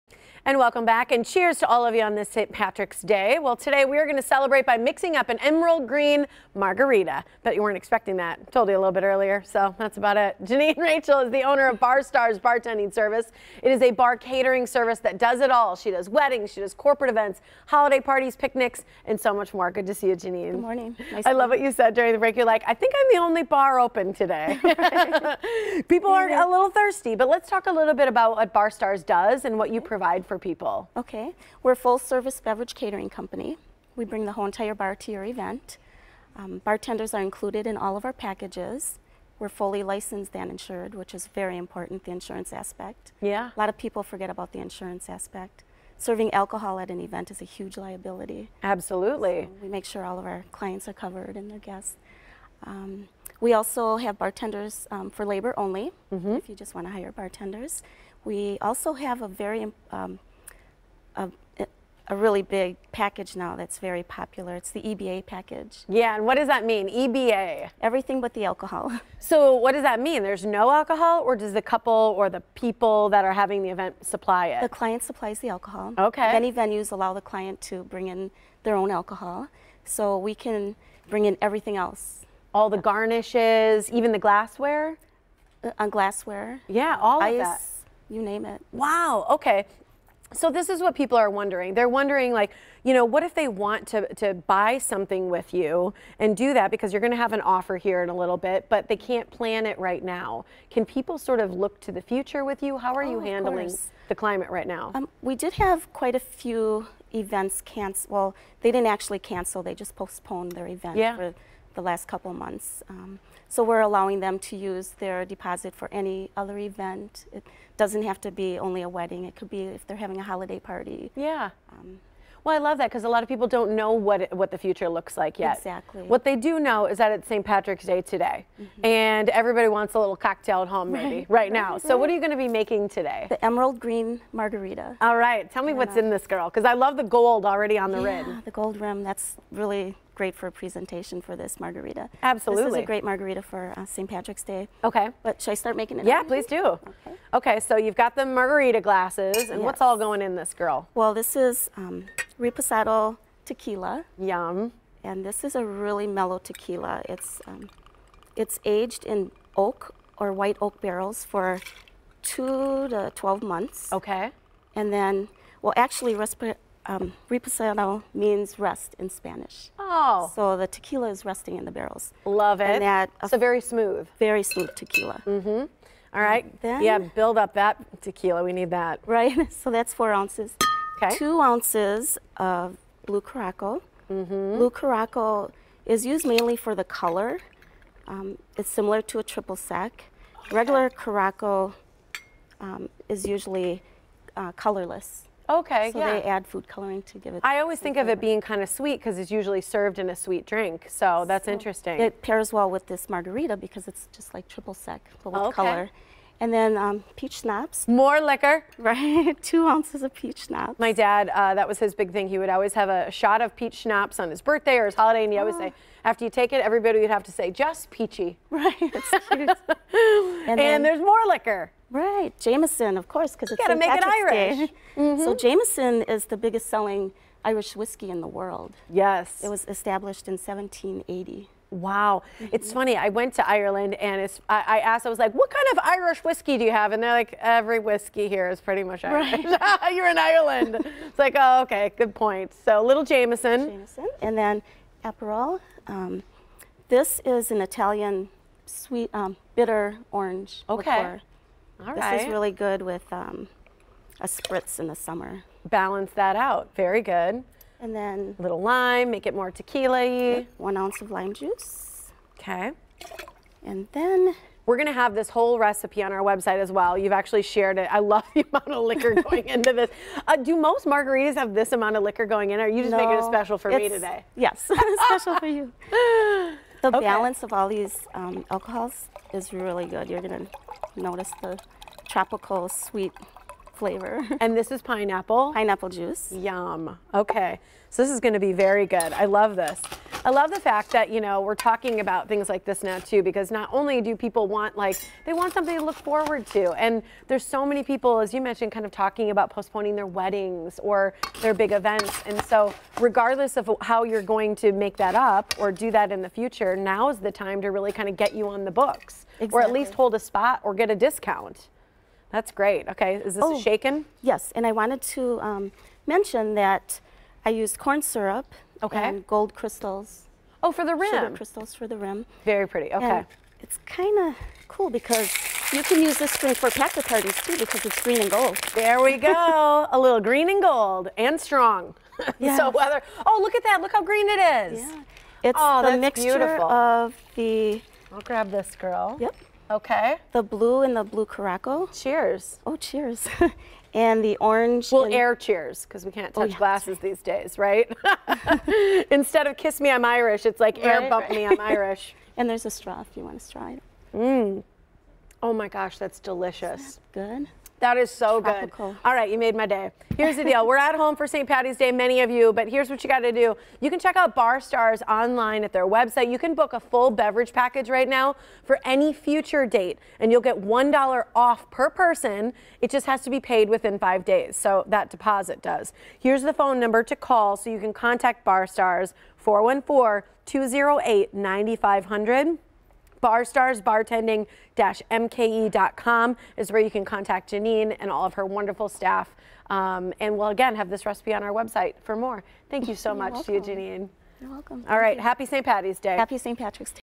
Celebrate St. Patrick's Day with a Signature Drink
Take the stress out of planning a party and let Bar Stars step in. Owner Janine Rajchel mixes up the perfect, festive beverage this Saint Patty's Day - an Emerald Green Margarita! She fills us in on all the services Bar Stars provides and how they can make any event truly unique to your needs.  Recipe for Emerald Green Margarita:  *Makes 2 - 4 drinks (depending on the size of glass)  Dip rim of glass in corn syrup and roll rim in gold sugar crystals.  4 oz Tequila (resposado) 2 oz Blue Curacao 2 oz Peach Schnapps 1 oz Jameson Irish Whiskey 1 oz Aperol  1 oz Rose Lime Juice 8 - 12 oz Pineapple Juice  Add ingredients to shaker - fill with ice - shake - strain into glass filled with crushed/shaved ice. Garnish with lime.  *Deposit must be paid within 5 days of booking for any future date.